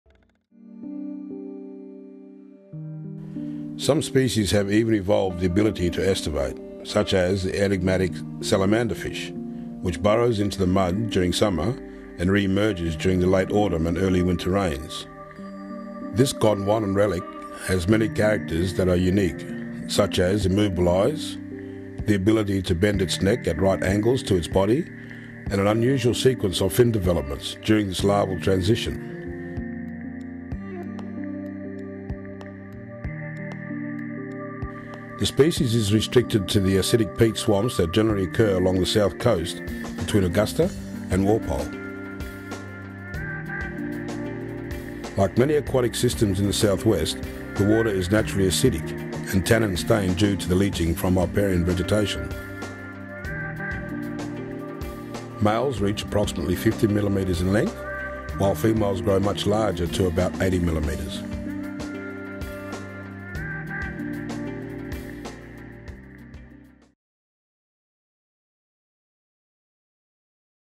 Film 1.2 NFF Salamanderfish
The native freshwater fishes of South -- Western Australia are highly endemic, meaning they are found nowhere else on earth and are a result of millions of years of isolation. This first of its kind film highlights these sometimes rare and endangered species in their natural habitat. Narrated by local Nyungar, Mr Joeseph Northover the film presents research information been gathered by the Murdoch University centre for fish and fisheries research over the last 20 years. The film also depicts the iconic south west freshwater environment and the pressures these fish face in climate change, increased abstraction and introduction of feral fish species. Keep watching through to the credits for the incredible fish rap.